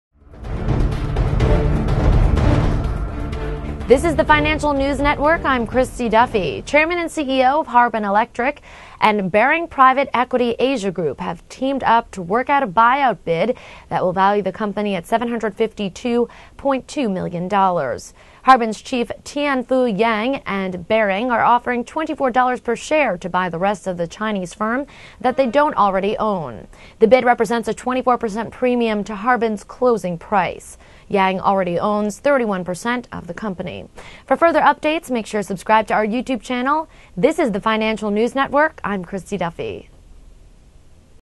Chairman, CEO of Harbin, Baring Private Equity Asia Team Up To Work On Buyout Bid
Chairman and CEO of Harbin Electric (NASDAQ:HRBN) and Baring Private Equity Asia Group have teamed up to work out a buyout bid that will value the company at $752.2 million.

Harbin's chief Tianfu Yang and Baring are offering $24 per share to buy the rest of the Chinese firm that they don't already own. The bid represents a 24% premium to Harbin's closing price.

Yang already owns 31% of the company.